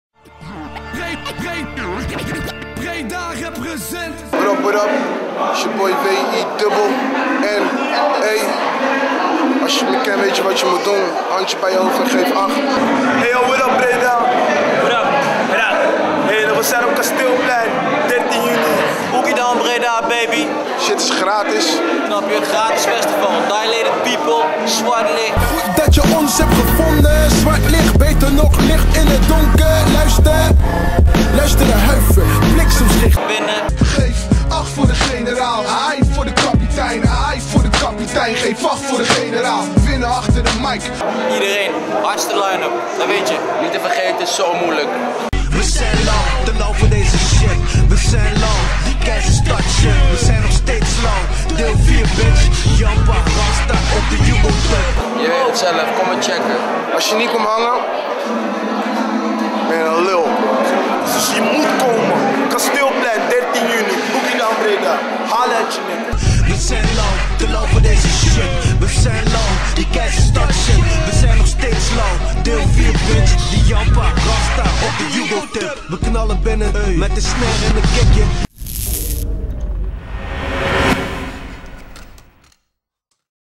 Boogiedown Breda TV 2009
De line-up voor de volgende Boogiedown Breda is bekend gemaakt.
Op zaterdag 13 juni treden op het Kasteelplein de volgende artiesten op :De headliner van het  Boogiedown Breda festival 2009 komt vanuit de westkust van de U.S.A.
Niemand minder dan de Dilated Peoples komen naar Breda voor een onvergetelijke hiphopshow.

Verder bestaat het programma uit de Rotterdammer Winne, de Nijmeegse Modo van Zo Moeilijk, de hiphoprevelatie Zwart Licht, de Keizer Punchline Jiggy Djé, D.A.C.er Diggy Dex en de Bossenaren Cartes & KleineJay. De lokale scene wordt vertegenwoordigd door Fotosynthese & aBREcaDAbra.
Winnaar van de Artquake contest, Schager Glorie uit Noord-Holland heeft de eer om het festival te openen. Vibecontrollers van de dag zijn DJs DNS & EZD (Shit is Bangin) en het geheel wordt gehost door Bashful & Deams.

Deze hele dag vol met optredens, 1on1 BBOY battles door Jammin on Beat, Graffiti-art & Skateboarding wordt mede mogelijk gemaakt door de Gemeente Breda, NHTV Breda, Gigant & Cream clothing.